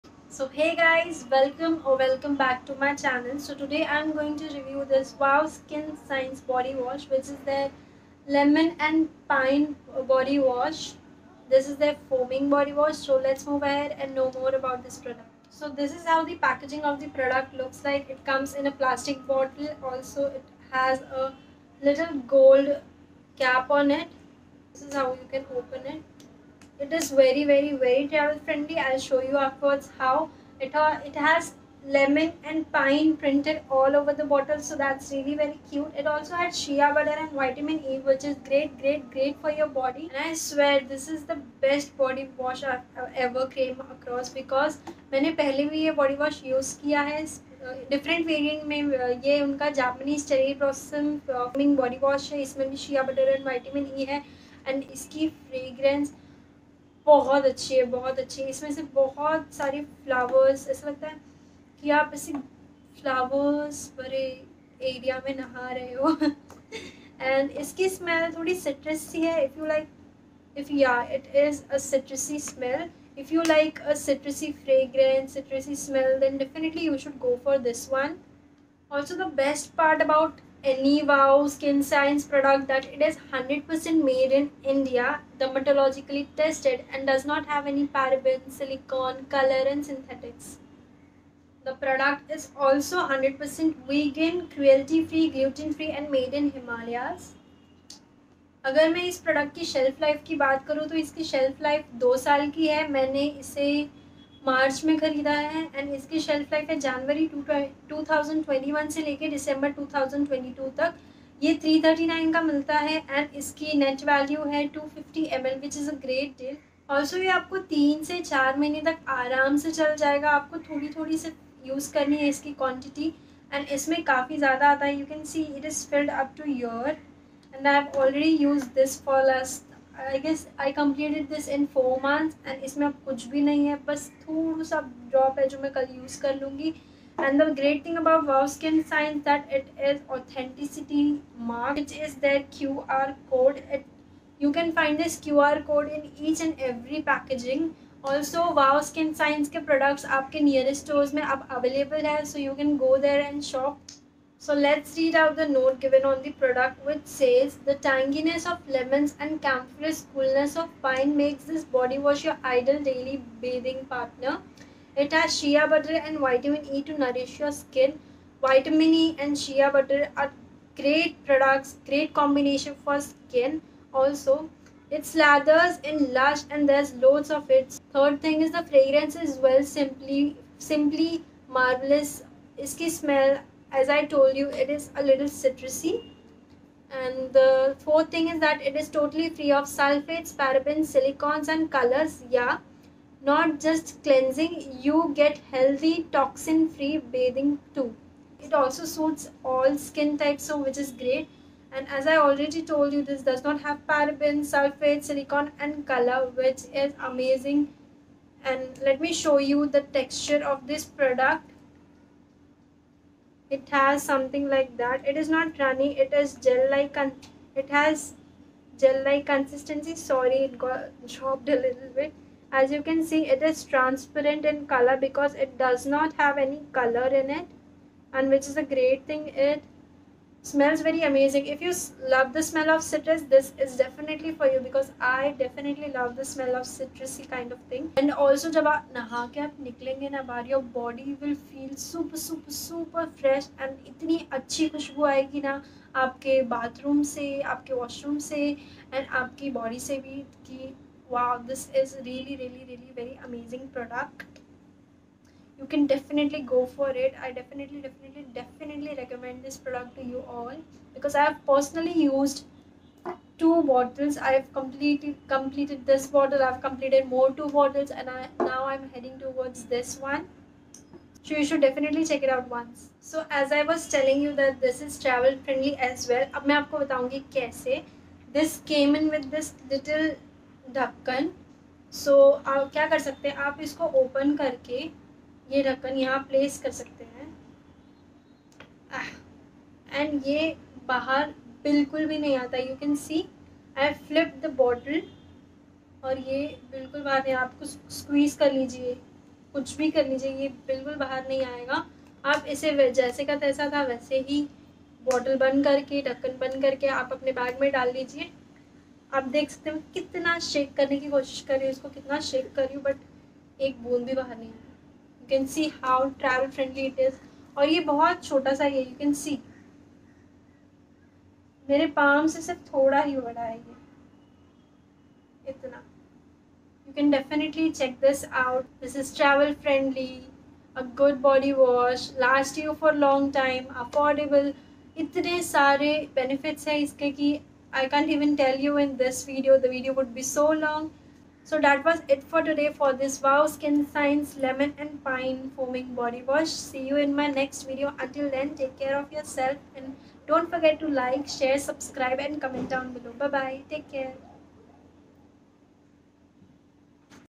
WOW skin science foaming bodywash | lemon & pine | Vinita Tiwari
Hello loves 💓
  This Body wash helps to clean Sweat, Dart particles, Excessive oils from your skin & body. this bodywash also moisturizers skin. this suits all skin types but it's best from dry skin people.

Ingredients: Shea Butter, Aloe Vera, Argan Oil, Vitamin E (Plant Based Ingredients).

Main Feature: This Bodywash cleans the skin deeply and provides provides nourishment which makes your skin feel soft and smooth.

Suitable: for all skin people.